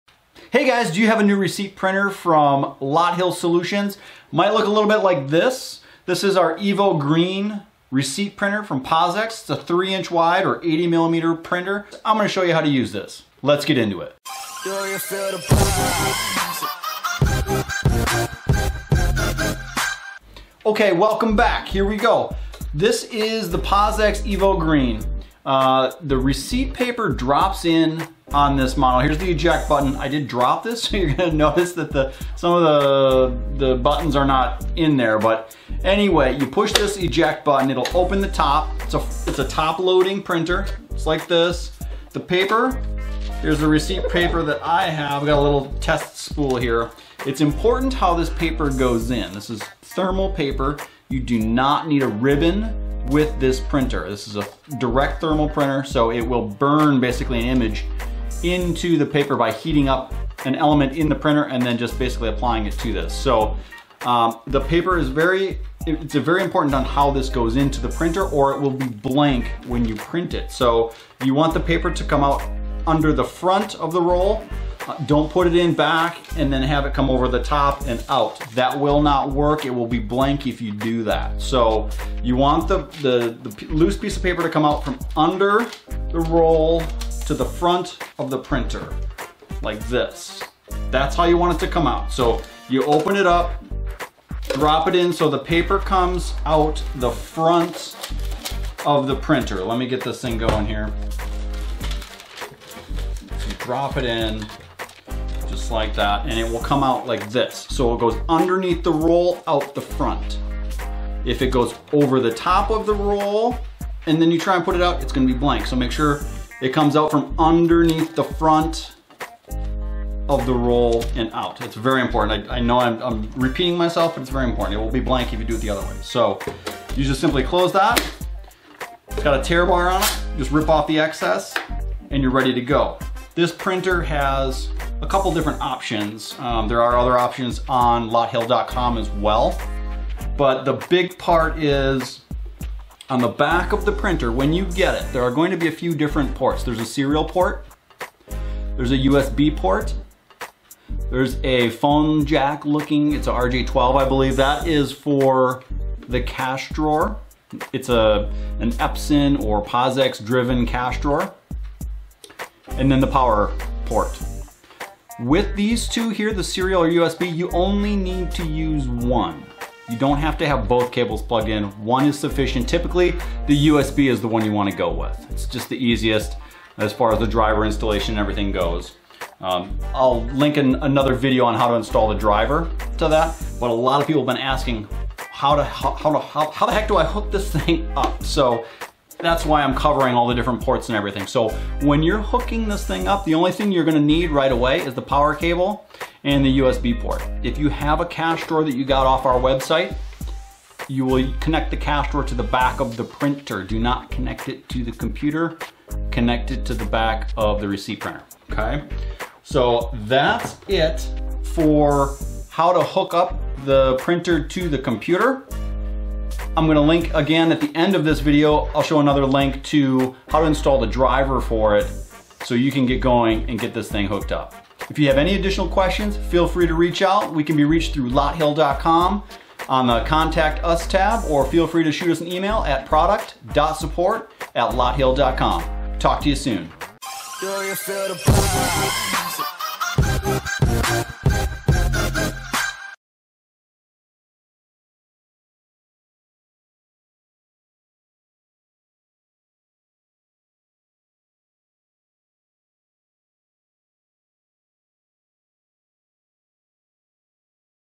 Thermal Receipt Printer: A Quick Lesson on Receipt Printer Basics
We discuss thermal receipt printer basics including how to load receipt paper, what cables are used, and how to connect a thermal receipt printer to your pos system or computer.

--- Products discussed in this video ---
- POS-X EVO Green Thermal Receipt Printer

- Canon T7i
- Rode VideoMic Pro+ microphone
- Edited w/ Corel VideoStudio Ultimate 2018